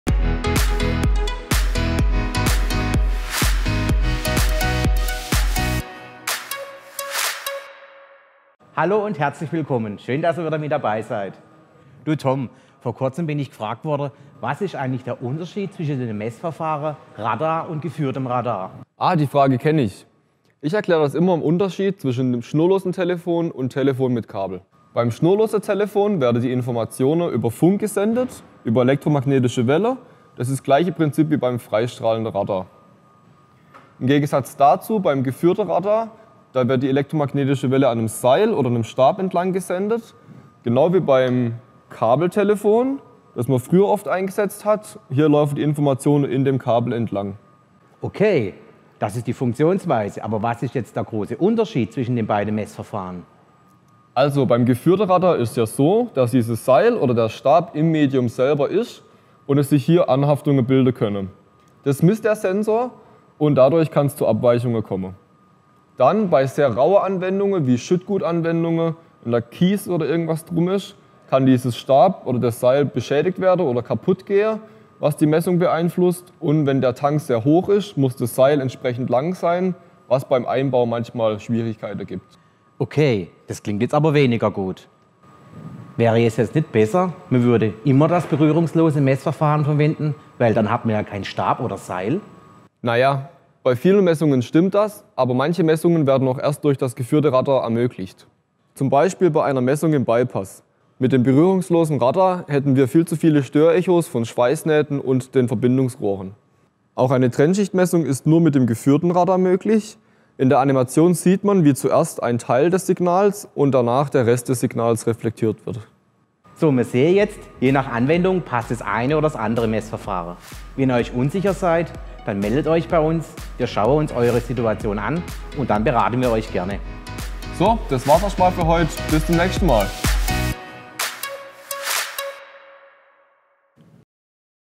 💡 Radar vs. Geführtes Radar (TDR) – Was sind die Unterschiede der Messverfahren? | VEGA talk
In der ersten Folge von VEGA talk geht es um die Messverfahren Radar und Geführtes Radar, auch bekannt als TDR (Time Domain Reflectometry = Laufzeitmessverfahren). Welche Sensoren eignen sich für welche Anwendung, wie funktioniert die Füllstand- und Trennschichtmessung und welche Vorteile bieten die beiden Messprinzipien?

Noch mehr Experten-Tipps und Neuheiten rund um Füllstand- und Druckmesstechnik?